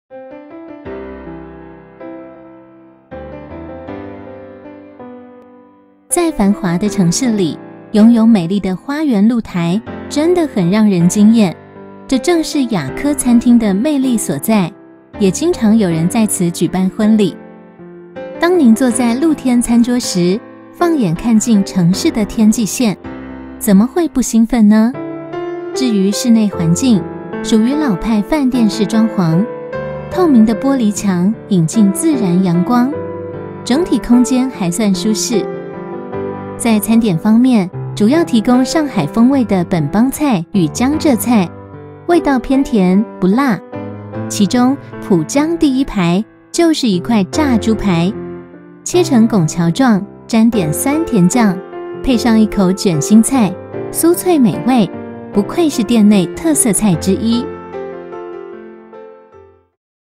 [黃浦區] 雅科(大光明店) (美食)
在繁華的城市裡，擁有美麗的花園露臺，真的很讓人驚艷，這正是雅科餐廳的魅力所在，也經常有人在此舉辦婚禮。當您坐在露天餐桌時，放眼看盡城市的天際線，怎麼會不興奮呢?至於室內環境，屬於老派飯店式裝潢，透明的玻璃牆引進自然陽光，整體空間還算舒適。在餐點方面，主要提供上海風味的本幫菜與江浙菜，味道偏甜、不辣；其中「浦江第一排」，就是一塊炸豬排，切成拱橋狀，沾點酸甜醬、配上一口捲心菜，酥脆美味，不愧是店內特色菜之一。